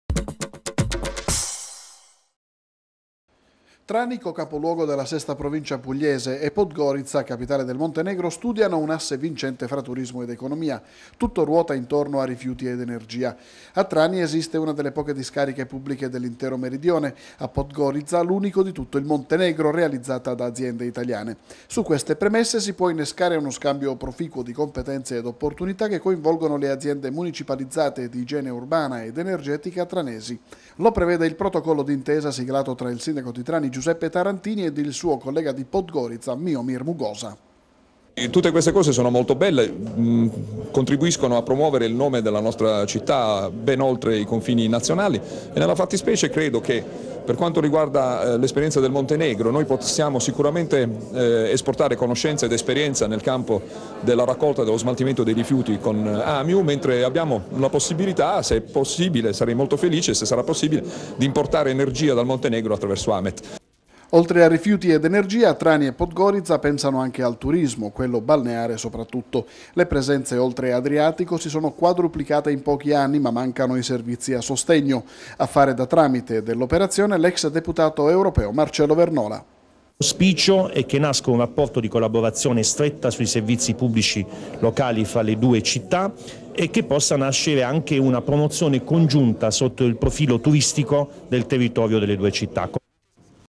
TG 26.06.10 Trani e Podgorica, protocollo sul turismo
Italia e Montenegro sempre più vicine, in nome della cultura, del turismo e del commercio. I sindaci di Podgorica, capitale del Montenegro, e Trani, co-capoluogo della sesta provincia pugliese, hanno siglato un protocollo d'intesa.